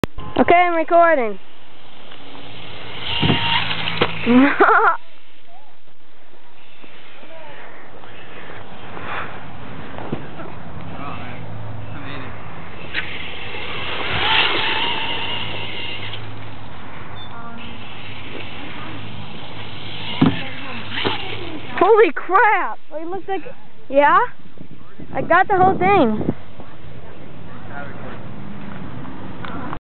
RC Jump
Bike Rap Jump Stampede Jumping With Slicks

Stampede jumping a bike rap with slicks on
Racing Stampede jumping with slicks on brushless motor,,

I have a NoVak super sport 5800 brushless motor in it. The car but for the rims is about all aluminun. If your going to get a RC car or truck make sure to get the best battery/s or your be sorry.